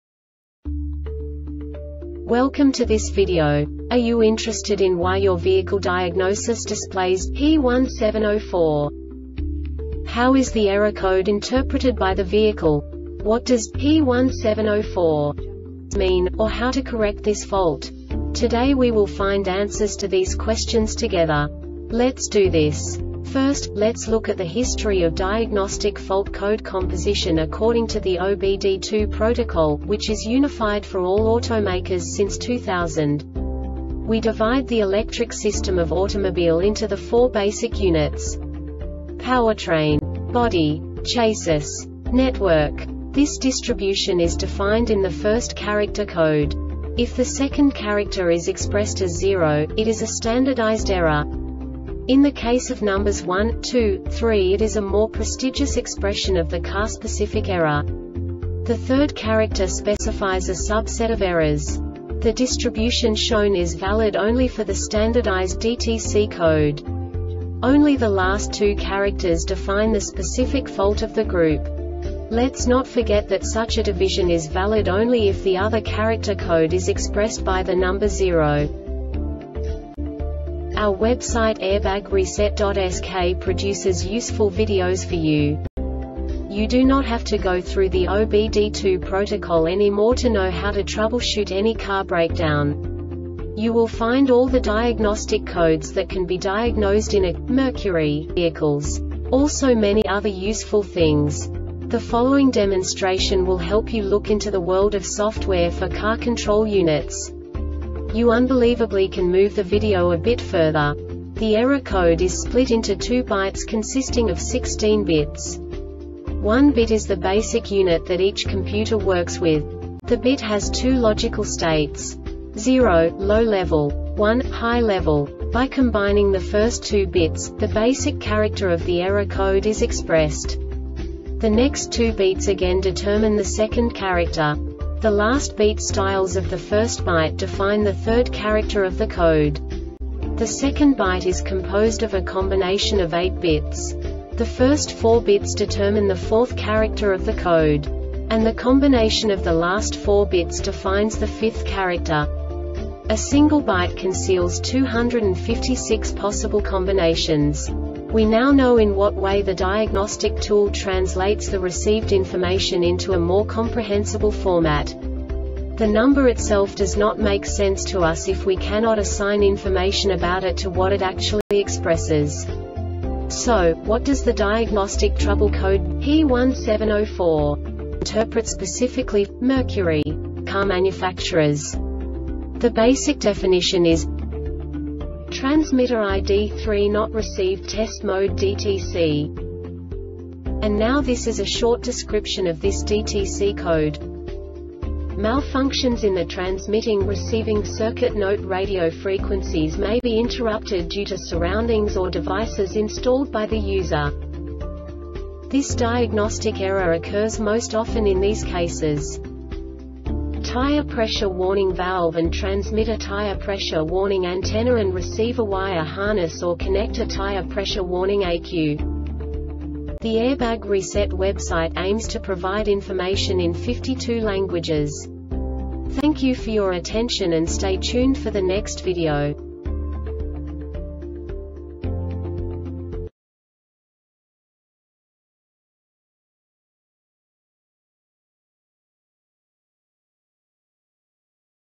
DTC Mercury P1704 Short Explanation
Contents:
0:21 Basic DTC analysis according to OBD2 protocol standard.
1:48 Insight into programming
2:58 A diagnostically understandable description of the Diagnostic Trouble Code
3:05 Basic error definition

Make
Mercury
Code
P1704
Definition
Transmitter ID3 not Received Test Mode DTC
Description
Malfunctions in the transmitting or receiving circuit  NOTE Radio frequencies may be interrupted due to surroundings or devices installed by the user
Cause
 Tire pressure warning valve and transmitter  Tire pressure warning antenna and receiver  Wire harness or connector  Tire pressure warning ECU